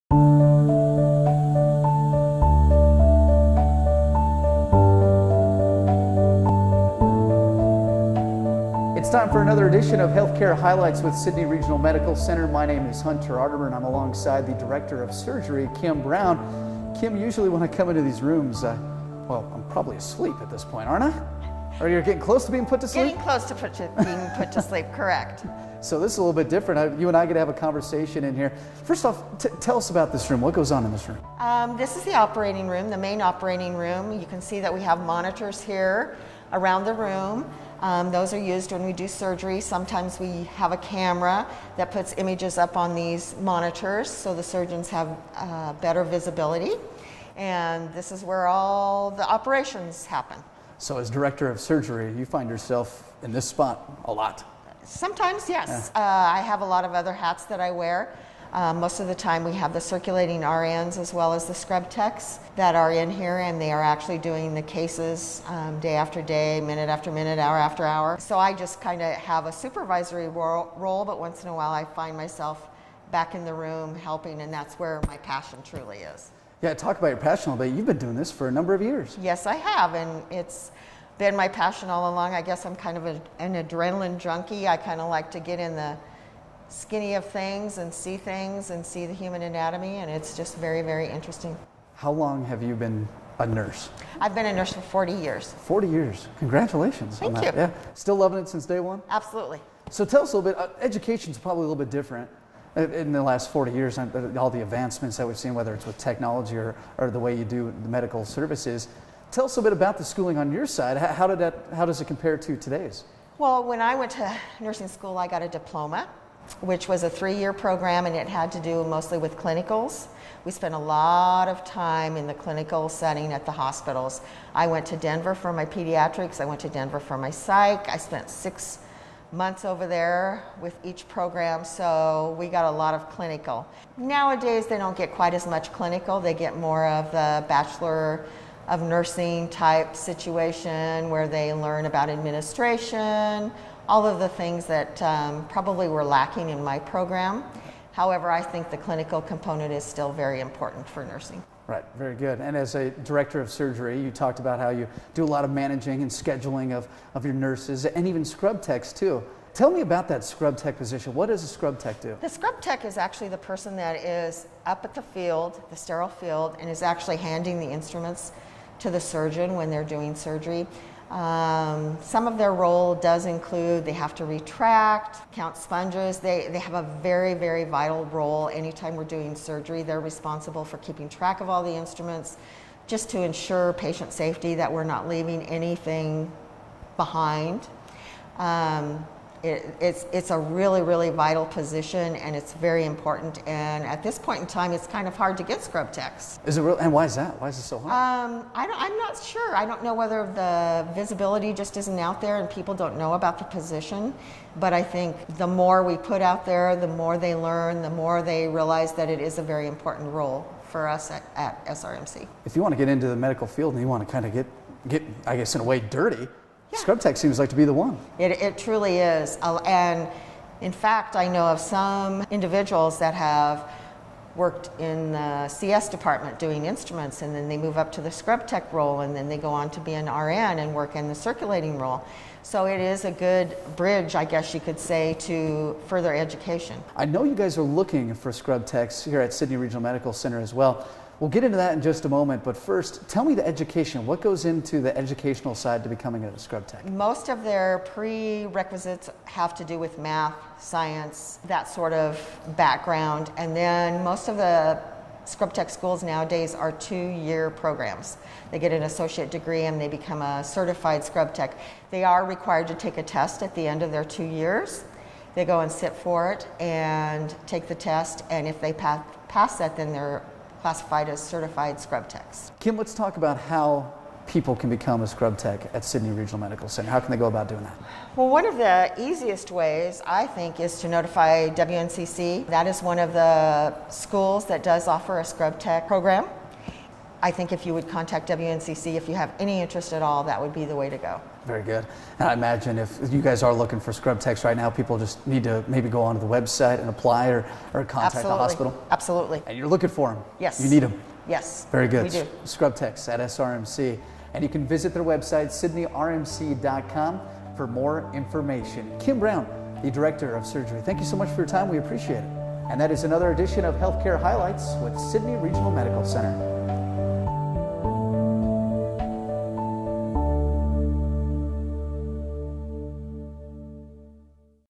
Healthcare Highlights - Surgical Services with Kim Brown
Healthcare Highlights was created to give patients and future healthcare workers an inside look into our facility, Sidney Regional Medical Center.